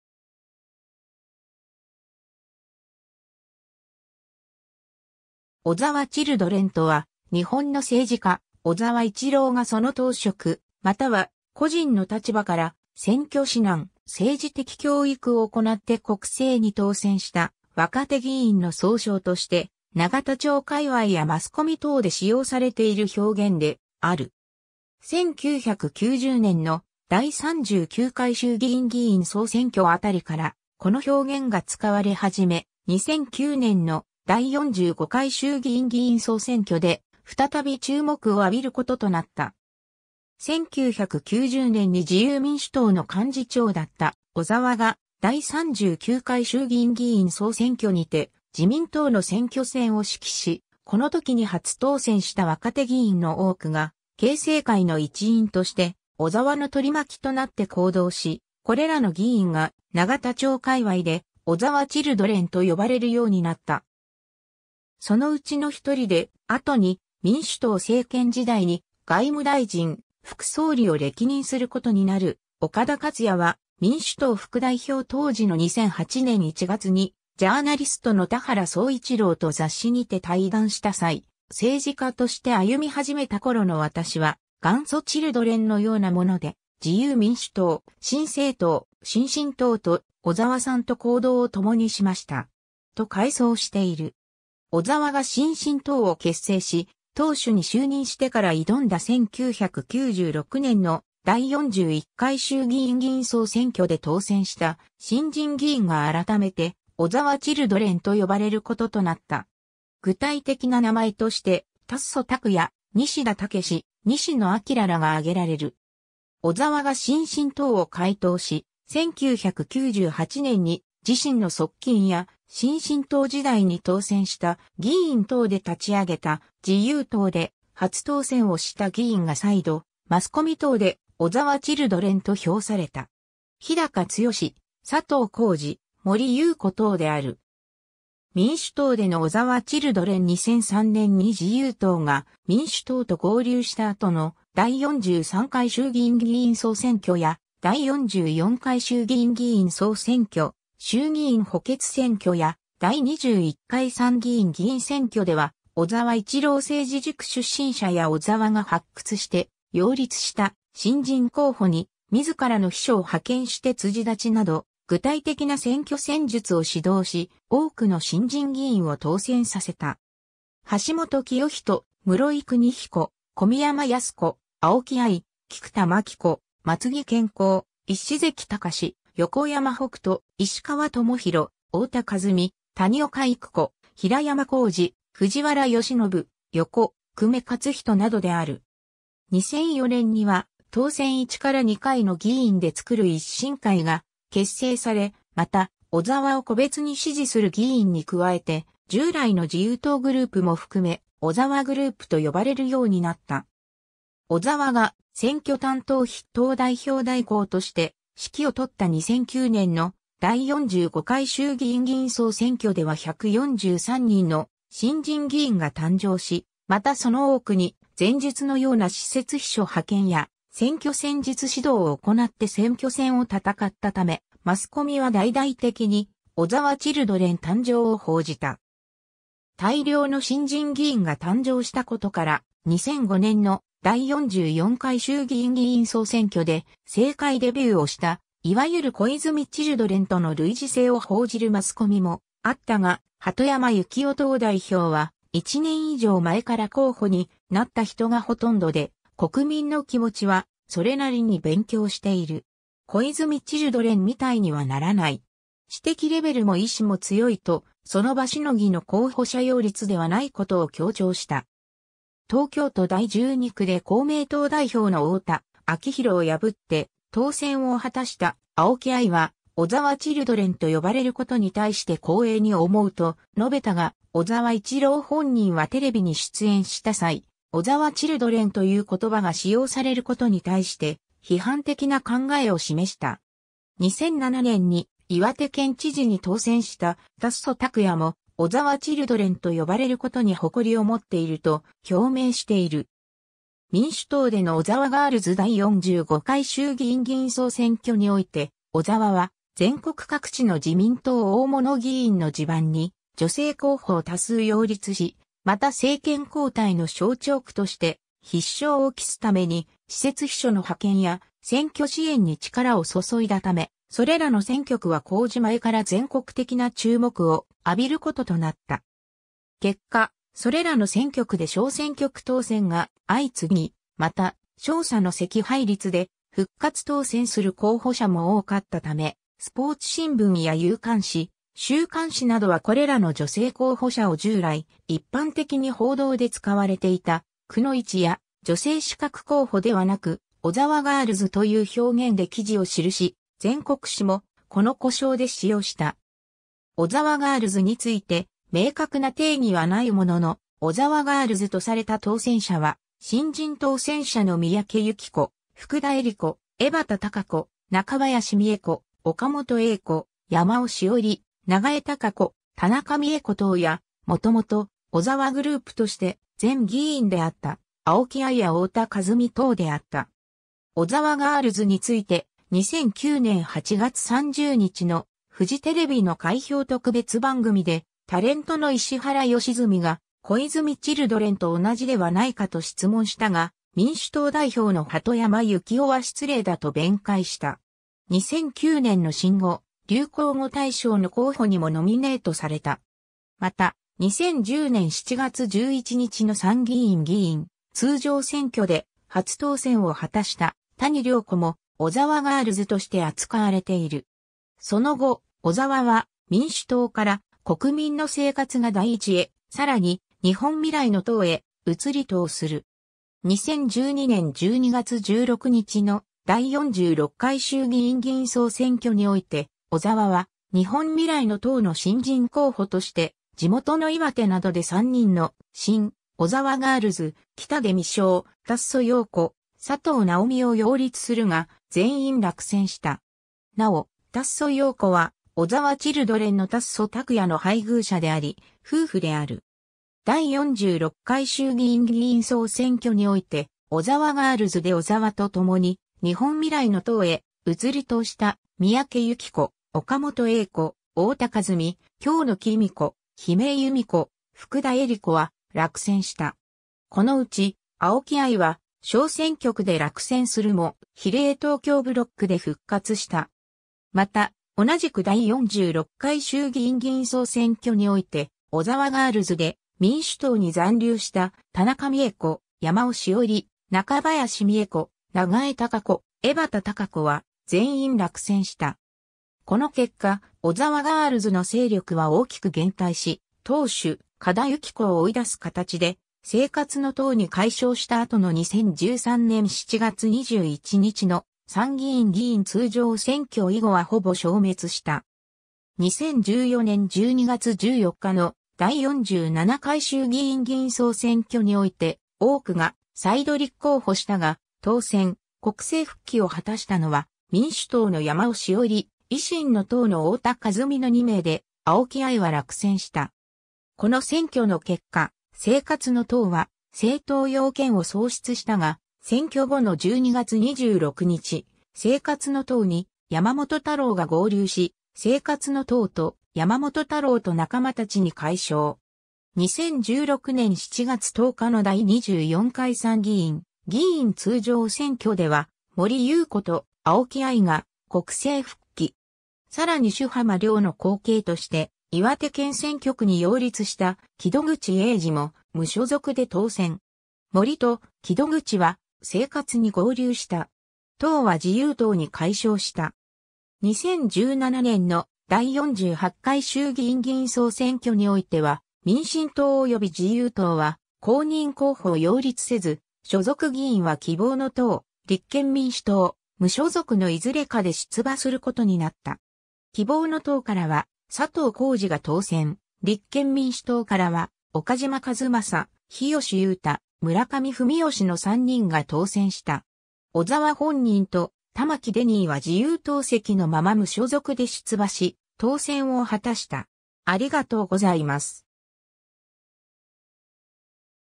小沢チルドレン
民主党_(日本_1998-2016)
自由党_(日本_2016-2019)
小沢チルドレン（おざわチルドレン）とは、日本の政治家小沢一郎がその党職または個人の立場から選挙指南、政治的教育を行って国政に当選した若手議員の総称として永田町界隈やマスコミ等で使用されている表現である。
1990年の第39回衆議院議員総選挙あたりからこの表現が使われ始め、2009年の第45回衆議院議員総選挙で再び注目を浴びる事となった。
 1990年に自由民主党の幹事長だった小沢が第39回衆議院議員総選挙にて自民党の選挙戦を指揮し、この時に初当選した若手議員の多くが経世会の一員として小沢の取り巻きとなって行動し、これらの議員が永田町界隈で「小沢チルドレン」と呼ばれるようになった。
 そのうちの1人で、後に民主党政権時代に外務大臣・副総理を歴任することになる岡田克也は、民主党副代表当時の2008年1月にジャーナリストの田原総一朗と雑誌にて対談した際、「政治家として歩み始めたころの私は、元祖チルドレンのようなもので、自由民主党、新生党、新進党と、小沢さんと行動をともにしました。」と回想している。
 小沢が新進党を結成し、党首に就任してから挑んだ1996年の第41回衆議院議員総選挙で当選した新人議員が改めて「小沢チルドレン」と呼ばれることとなった。
具体的な名前として、達増拓也、西田猛、西野陽らが挙げられる。
 小沢が新進党を解党し、1998年に自身の側近や新進党時代に当選した議員等で立ち上げた自由党で初当選をした議員が再度、マスコミ等で「小沢チルドレン」と評された。
樋高剛、佐藤公治、森ゆうこ等である。
 民主党での小沢チルドレン 2003年に自由党が民主党と合流した後の第43回衆議院議員総選挙や第44回衆議院議員総選挙、衆議院補欠選挙や第21回参議院議員選挙では、小沢一郎政治塾出身者 や小沢が発掘して擁立した新人候補に自らの秘書を派遣して辻立ちなど具体的な選挙戦術を指導し、多くの新人議員を当選させた。
橋本清仁、室井邦彦、小宮山泰子、青木愛、菊田真紀子、松木謙公、石関貴史、横山北斗、石川知裕、太田和美、谷岡郁子、平山幸司、藤原良信、横粂勝仁などである。
2004年には当選1～2回の議員で作る一新会が結成され、また小沢を個別に支持する議員に加えて従来の自由党グループも含め、小沢グループと呼ばれるようになった。
 小沢が選挙担当筆頭代表代行として指揮を執った2009年の第45回衆議院議員総選挙では143人の新人議員が誕生し、またその多くに前述のような私設秘書派遣や選挙戦術指導を行って選挙戦を戦ったため、マスコミは大々的に「小沢チルドレン」誕生を報じた。
 大量の新人議員が誕生したことから、2005年の第44回衆議院議員総選挙で政界デビューをしたいわゆる小泉チルドレン（83会）との類似性を報じるマスコミもあったが、鳩山由紀夫党代表は「1年以上前から候補になった人がほとんどで、国民の気持ちはそれなりに勉強している。小泉チルドレンみたいにはならない。知的レベルも意志も強い」と、その場凌ぎの候補者擁立ではないことを強調した。
 東京都第12区で公明党代表の太田昭宏を破って当選を果たした青木愛は「小沢チルドレンと呼ばれることに対して光栄に思う」と述べたが、小沢一郎本人はテレビに出演した際、小沢チルドレンという言葉が使用されることに対して批判的な考えを示した。
 2007年に岩手県知事に当選した達増拓也も小沢チルドレンと呼ばれることに誇りを持っていると表明している。
 民主党での小沢ガールズ 第45回衆議院議員総選挙において、小沢は全国各地の自民党大物議員の地盤に女性候補を多数擁立し、また政権交代の象徴区として必勝を期すために私設秘書の派遣や選挙支援に力を注いだため、それらの選挙区は公示前から全国的な注目を浴びることとなった。
結果、それらの選挙区で小選挙区当選が相次ぎ、また小差の惜敗率で復活当選する候補者も多かったため、スポーツ新聞や夕刊紙、週刊誌などはこれらの女性候補者を従来一般的に報道で使われていた「くノ一」や「女性刺客候補」ではなく「小沢ガールズ」という表現で記事を記し、全国紙もこの呼称で使用した。
「小沢ガールズ」について明確な定義はないものの、「小沢ガールズ」とされた当選者は、新人当選者の三宅雪子、福田衣里子、江端貴子、中林美恵子、岡本英子、山尾志桜里、永江孝子、田中美絵子 等や、元々小沢グループとして前議員であった青木愛 や太田和美 等であった。
 小沢ガールズ...